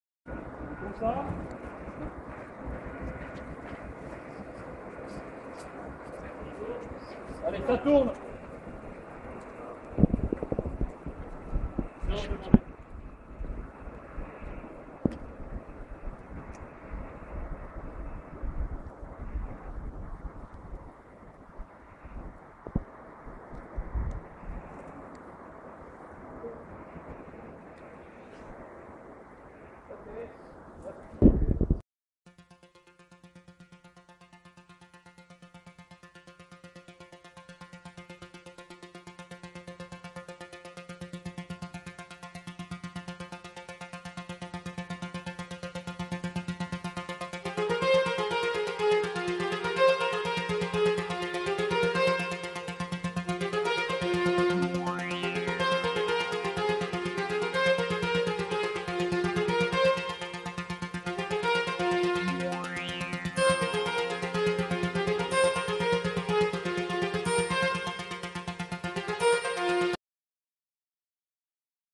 Tournage au Grand Dunkerque du film Arrêter moi sous la conduite Jean Paul LILENFELD.
Tournage au Grand Dunkerque sous la conduite du réalisAteur Jean Paul LILIENFELD du film "Arrêter moi" avec la participation de MIOU-MIOU et Sophie MARCEAU dont la sortie est programmée pour Octobre 2013.